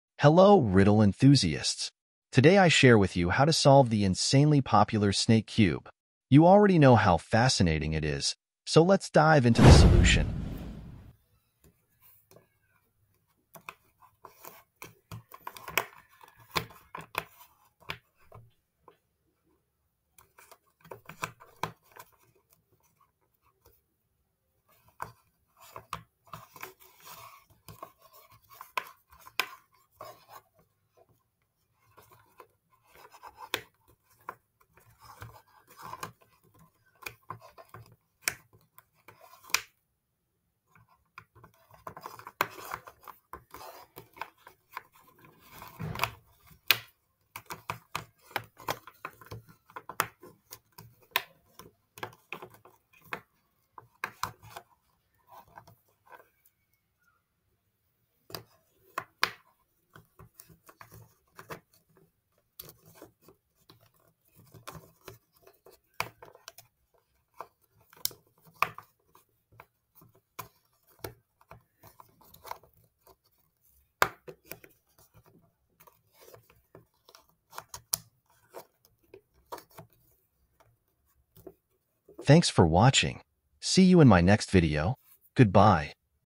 SNAKE CUBE - SOLUTION
Have you ever wondered the solution of the snake cube? For some of you it is more difficult than the Rubik's cube as it unfolds onto three dimensions! It's an interesting riddle to explore! Have fun!